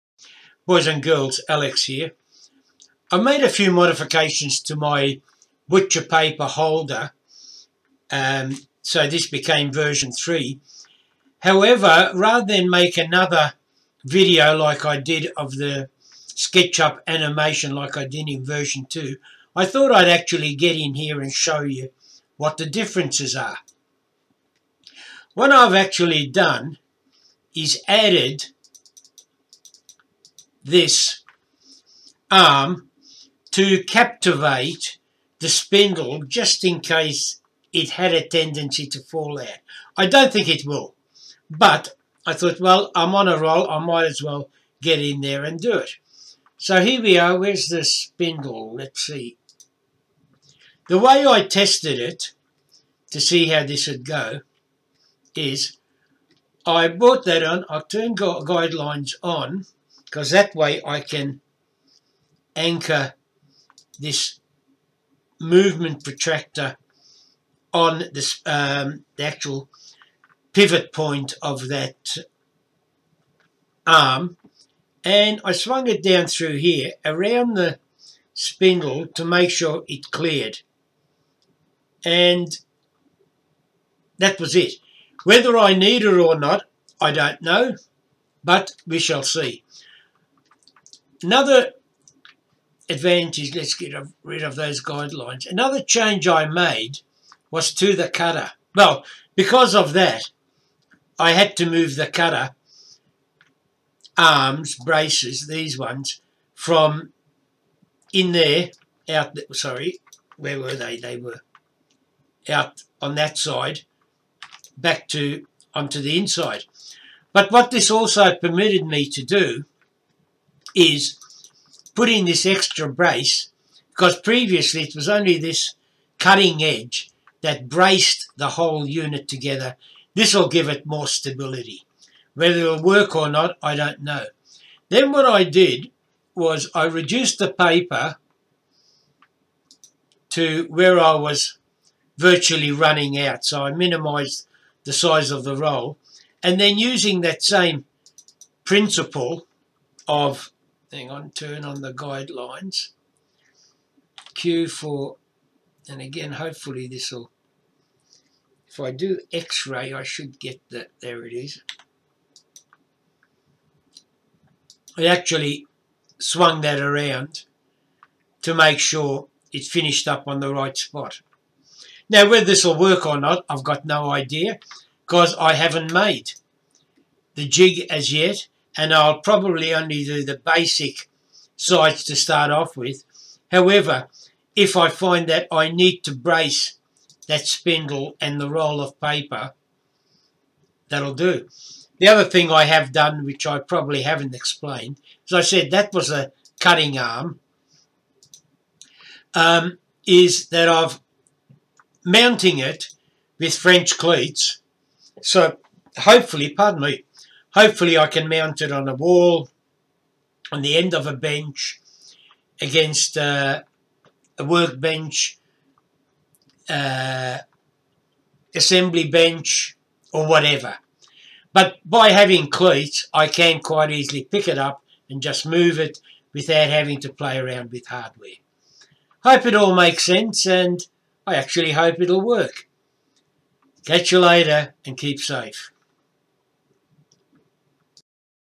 Butcher Paper Holder V2 (SketchUp modifications)
Boys and Girls,  JUst after publishing my previous BPH V2 Sketchup animation video, I thought about captivating the spindle.

Not having built the jig as yet, I have no idea whether it will be needed or not... but thought while I'm there it's easier to modify now than come back later if needed.

I thought a video of the modifications might serve better than another animation export.